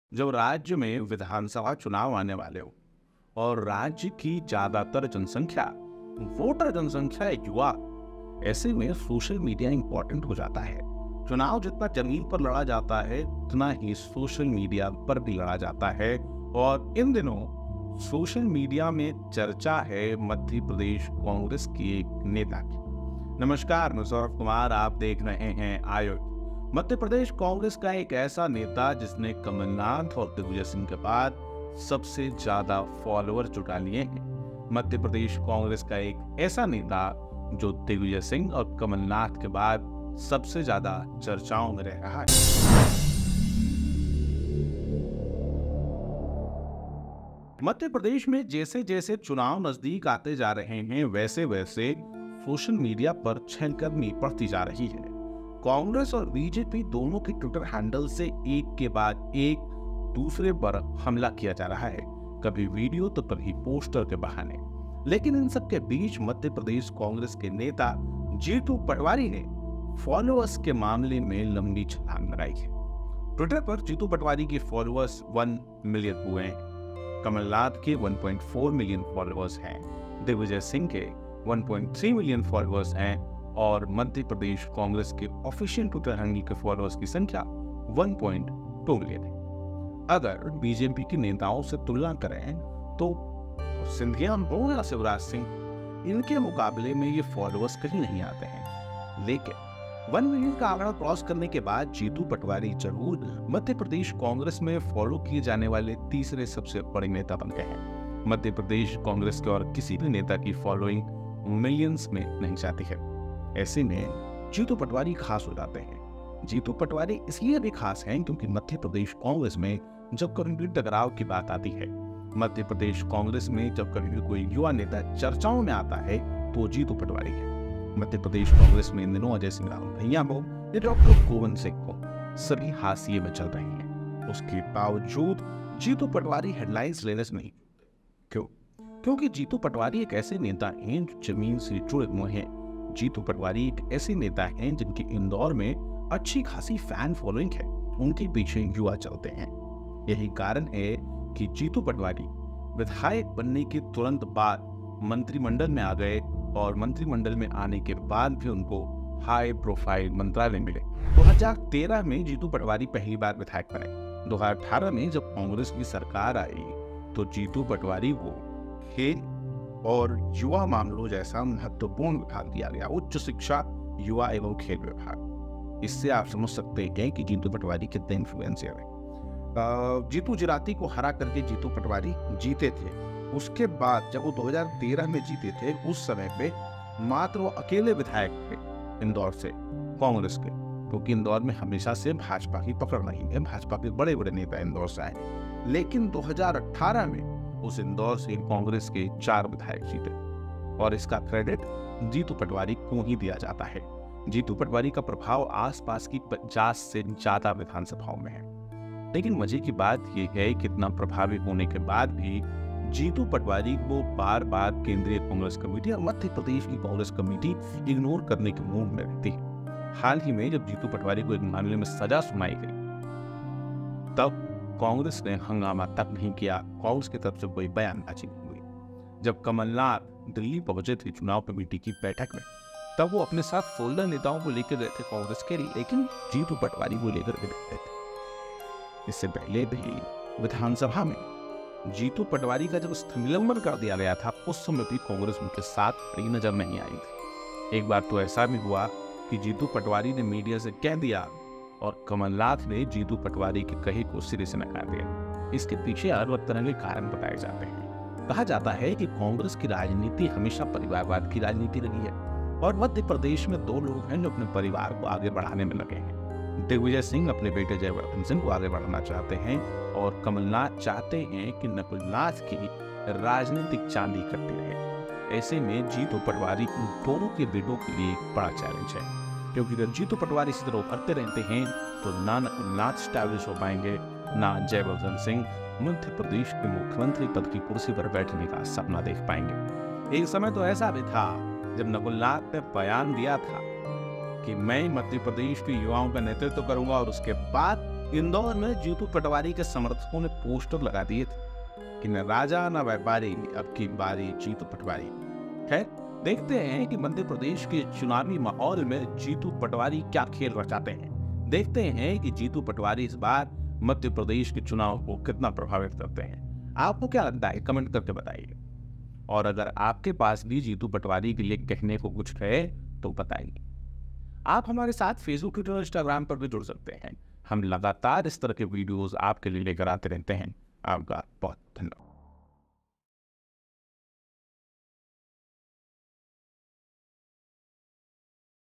Jitu Patwari No 3 in Congress : Kamal nath, Digvijay से बस एक कदम पीछे !
चुनावों के इस दौर में जितनी ज़रूरी जमीन है, उतनी हीं सोशल मीडिया और कांग्रेस के नेता जीतू पटवारी सोशल मीडिया में बाजी मार गए हैं। ट्विटर पर पटवारी दिग्विजय सिंह और कमलनाथ के बाद नंबर 3 हो गए हैं। देखिए क्यों खास है ये

Social media and Congress leader Jitu Patwari have won the battle in social media as much as the land is important in this era of elections. Patwari has become number 3 on Twitter after Digvijay Singh and Kamal Nath. see why it's special